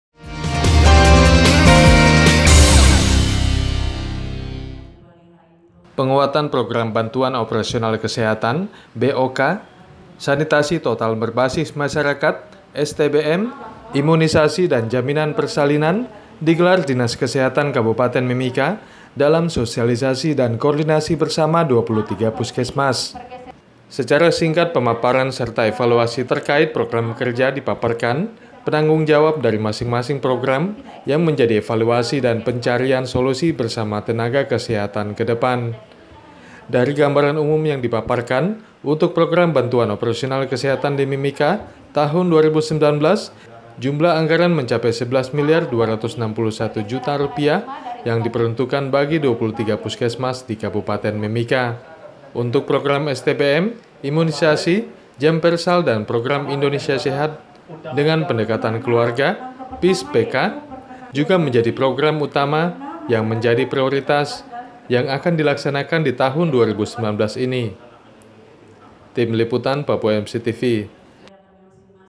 DINKES MIMIKA SOSIALISASI KOORDINASI PENGUATAN 4 PROGRAM TAHUN 2019
Penguatan program bantuan operasional kesehatan (bok) / sanitasi total berbasis masyarakat (stbm)/ imunisasi dan/ jaminan persalinan digelar dinas kesehatan kabupaten mimika dalam sosialisasi dan koordinasi bersama 23 puskesmas.